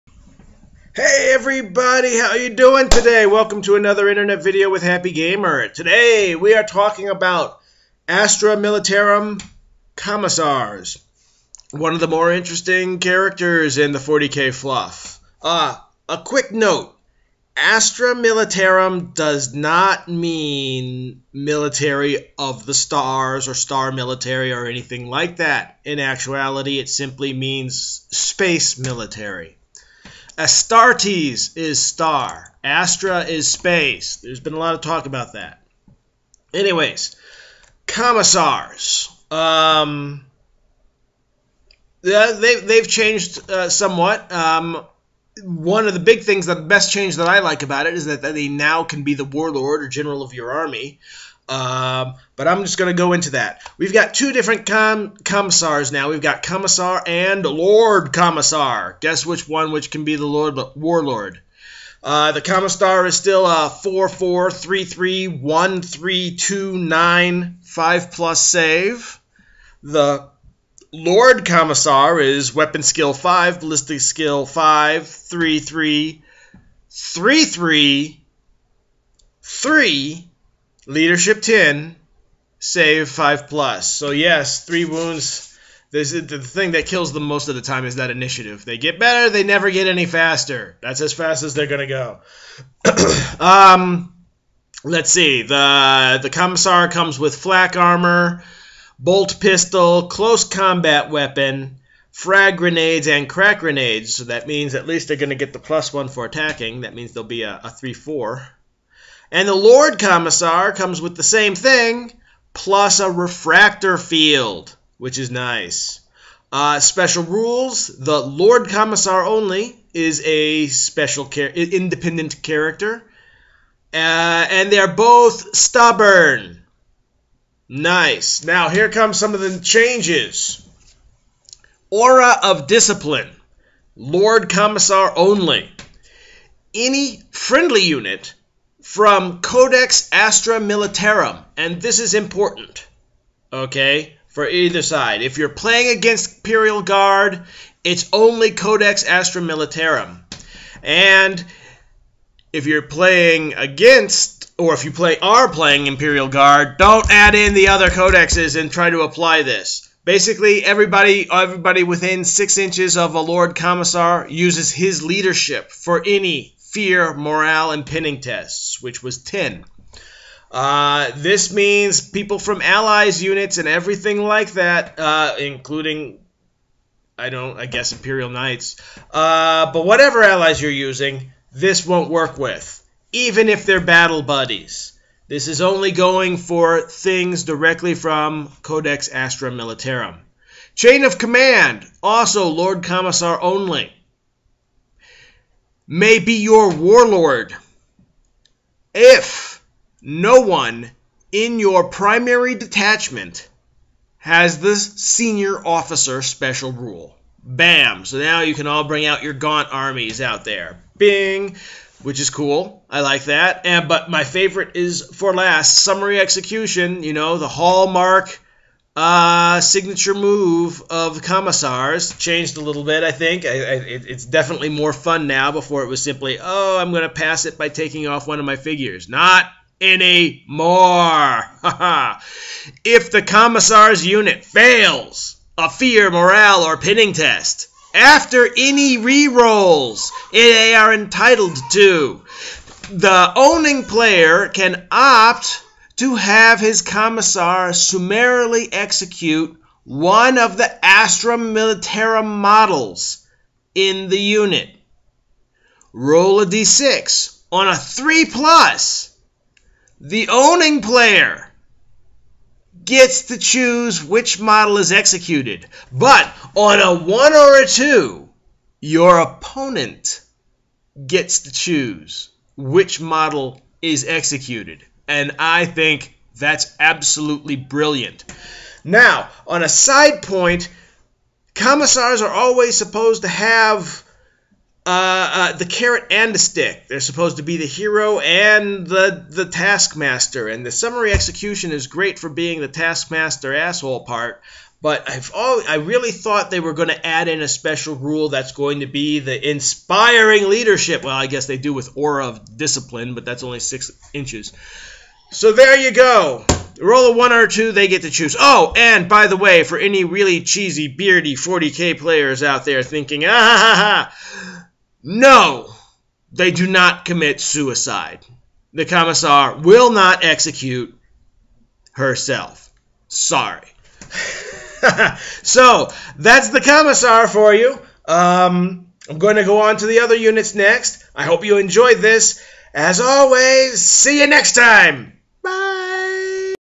ASTRA MILITARUM RULES: COMMISAR - Warhammer 40k
An overview of the Commissar from Astra Militarum.